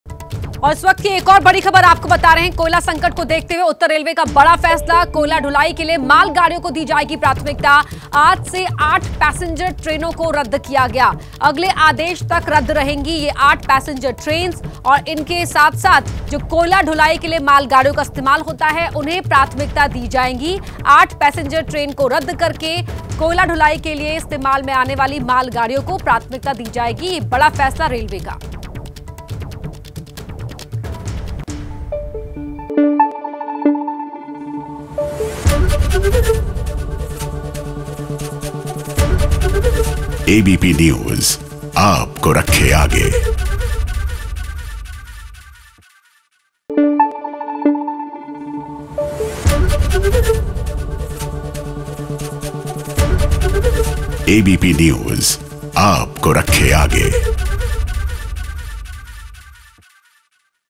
कोयला संकट को देखते हुए रेलवे ने लिया बहुत बड़ा फैसला
कोयला संकट को देखते हुए उत्तर रेलवे का बड़ा फैसला. .कोयला ढुलाई के लिए माल गाड़ियों को दी जाएगी प्राथमिकता...आज से 8 पैसेंजर ट्रेनों को किया रद्द. अगले आदेश तक रद्द रहेंगी पैसेंजर ट्रेनें.